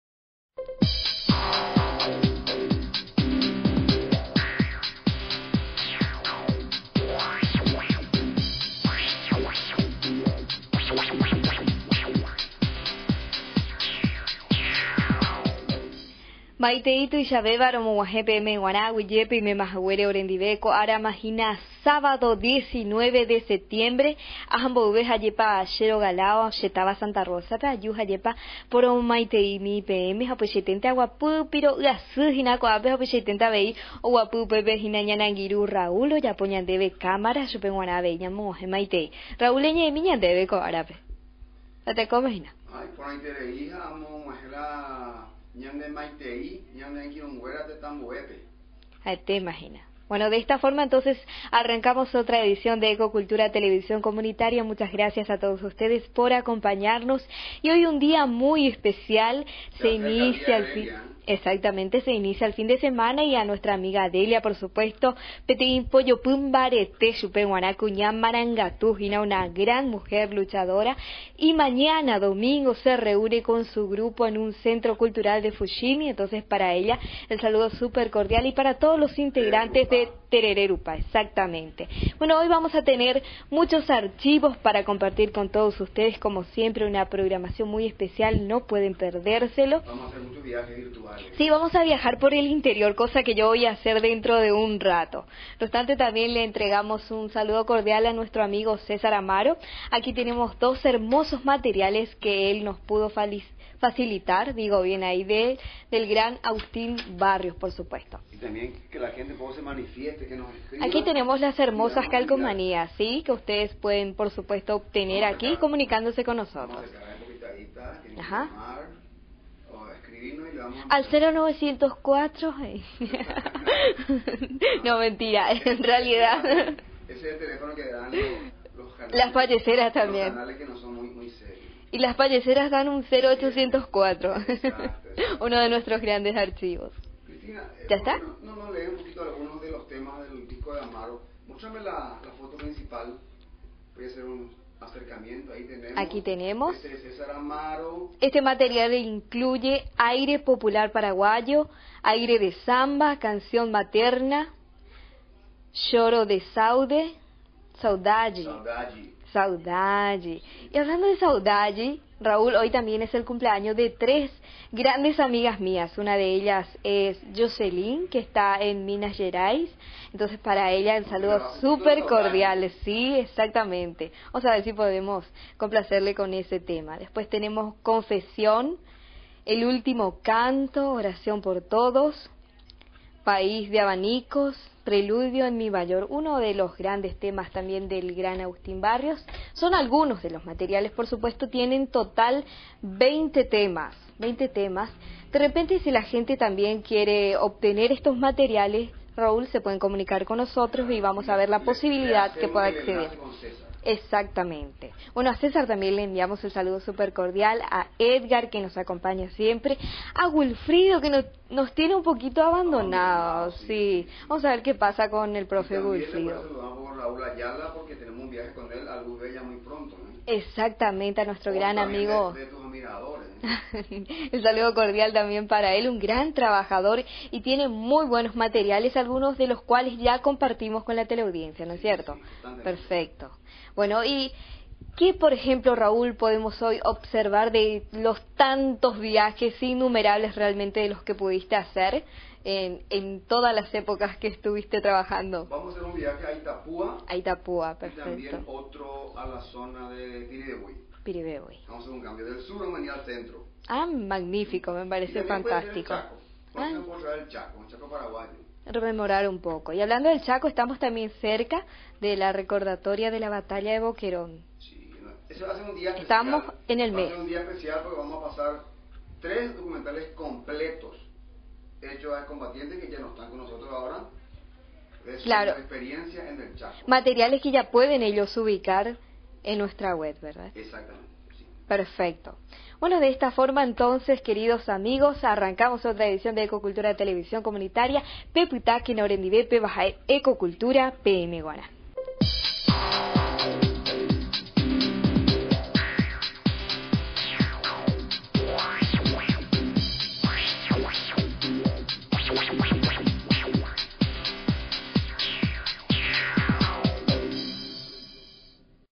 Sábado 19 con Cristina Benítez
Antes se su viaje de todos los sabados a su TAVA Santa Rosa Misiones, CB, nos alegra la jornada con su presencia. Disfrutemos de su guaraní, no sea que se la lleven al exterior. estamos recibiendo propuestas!!!